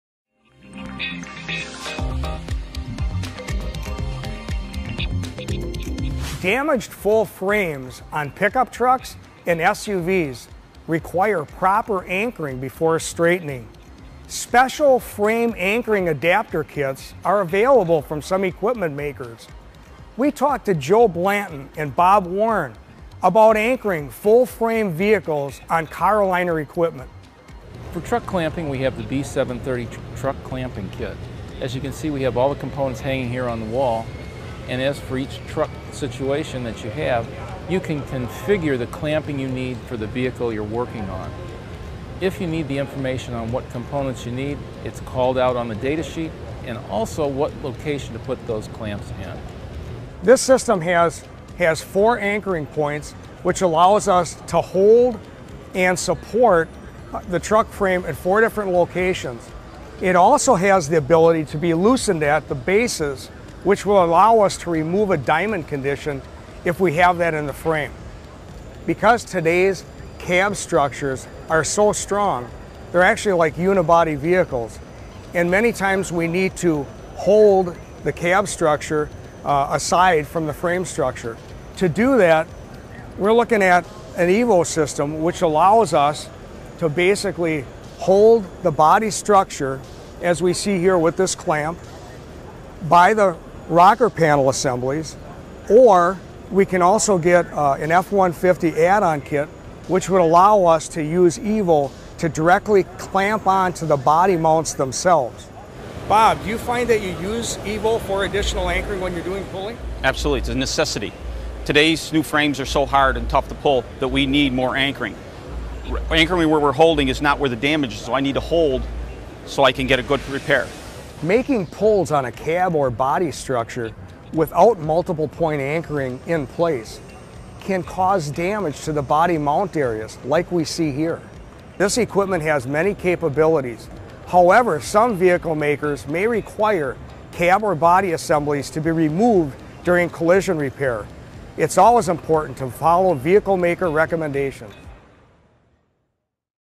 Car-O-Liner Truck Clamping
The Car-O-Liner® Truck Clamping and Support System allows you to anchor and repair every compact truck, pickup, van and sport utility vehicle – with holding power that far exceeds pulling requirements.

Damaged full frames on pickup trucks and SUV's require proper anchoring before straightening special frame anchoring adapter kits are available from some equipment makers we talked to Joe Blanton and Bob Warren about anchoring full-frame vehicles on
Car-O-Liner equipment.  For truck clamping we have to B730 truck clamping kit as you can see we have all components hanging here on wall and as for each truck situation that you have you can configure the clamping you need for the vehicle you're working on. If you need the information on what components you need it’s called out on the data sheet and also what location to put those clamps in.  This system has four anchoring points, which allows us to hold and support the truck frame at four different locations it also has the ability to be loosened at the bases which will allow us to remove a diamond condition if we have that in the frame.
Because today's cam structures are so strong they're actually like unibody vehicles and many times we need to hold the cab structure aside from the frame structure to do that we're looking at EVO system which allows us to basically hold the body structure as we see here with this clamp by the rocker panel assemblies or we can also get are an F 150 add-on kit which will allow us to use EVO to directly clamp onto the body mounts themselves. Bob you find that you use EVO for additional anchoring when you are doing pulling? Absolutely it’s a necessity today's new frames are so hard and tough the pull that we need more anchoring.  Anchoring were we are holding is not where the damage so I need to hold so I can get a good repair. Making pulls on a cab or body structure without multiple point anchoring in place can cause damage to the body mount areas like we see here.  This equipment has many capabilities however some vehicle makers may require cab or body assemblies to be removed during collision repair it's always important to follow vehicle maker recommendation.